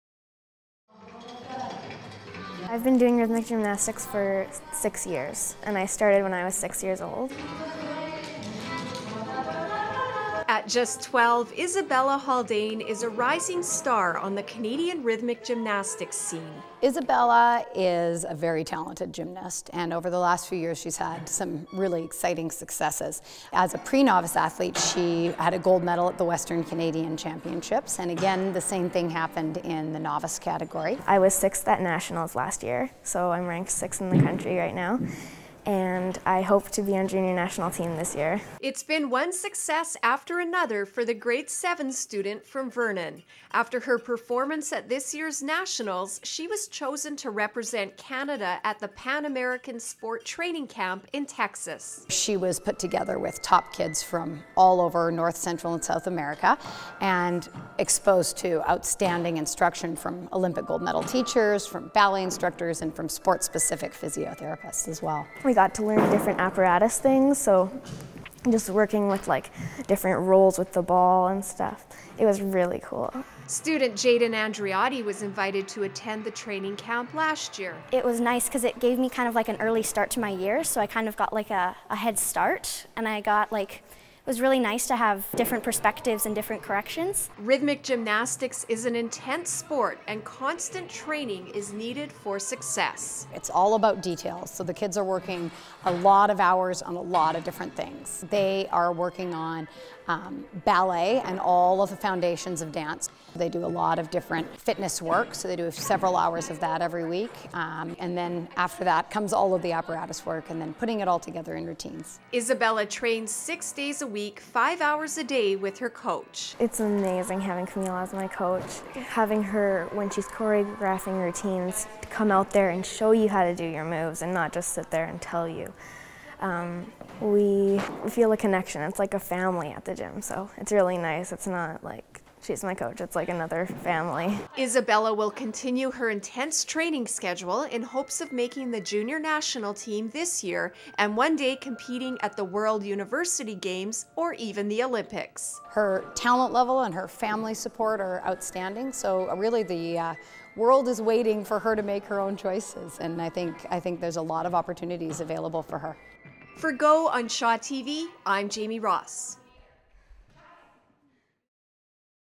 A Rising Star
Isabella Haldane is earning a name for herself on the Canadian Rhythmic Gymnastics scene, and she just received a very special honour.